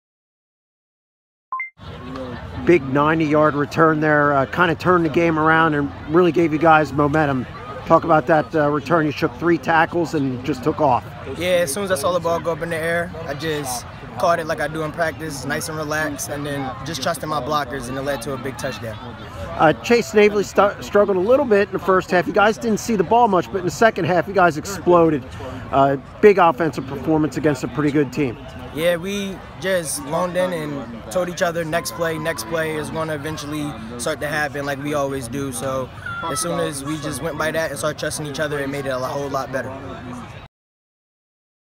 Middletown's Tyreer Mills
Middletown' Tyreer Mills talks about his 90-yard punt return for a touchdown and the Blue Raiders' explosive offense.